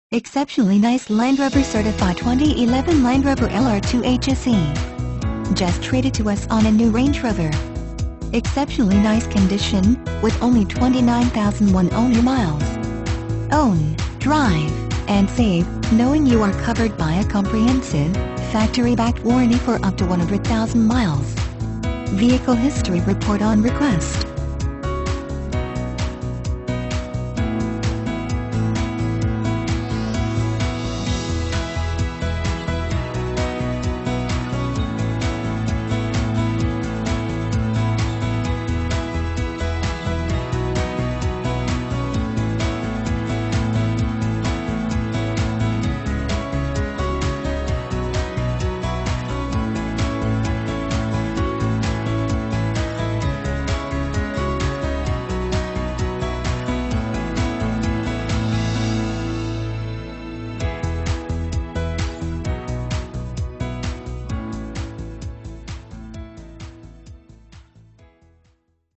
2011 Land Rover LR2 L160065A
2011 LAND ROVER LR2 Merritt Island, FL
Stock# L160065A

Island Lincoln
1850 East Merritt Island Cswy

Exceptionally Nice Land Rover Certified 2011 Land Rover LR2 HSE! Just Traded to Us on a New Range Rover! Exceptionally Nice Condition, with Only 29000 1-Owner Miles! Own, Drive, and Save, Knowing You are Covered by a Comprehensive, Factory Backed Warranty For Up to 100,000 Miles! Vehicle History Report on Request